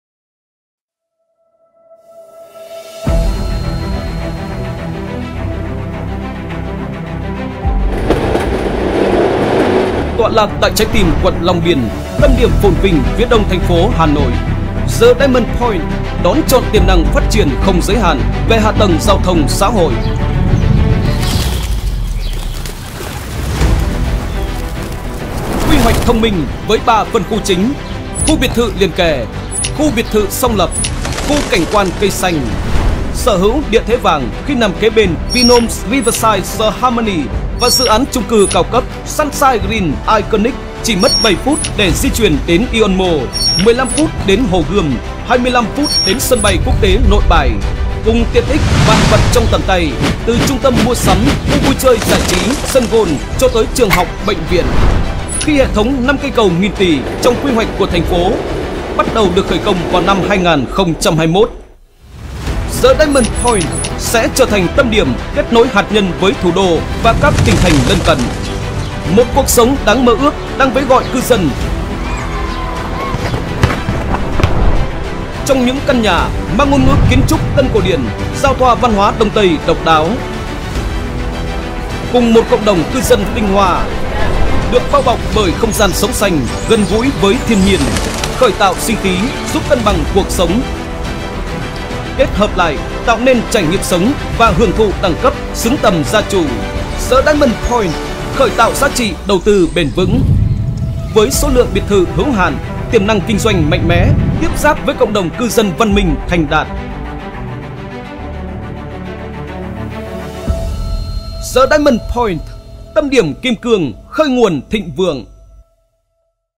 Video giới thiệu dự án The Diamond Point Hà Nội - CAFELAND.VN
Dự án khu biệt thự, nhà phố liền kề The Diamond Point Hà Nội tọa lạc ở đường Nguyễn Lam, phường Phúc Đồng, quận Long Biên, thành phố Hà Nội.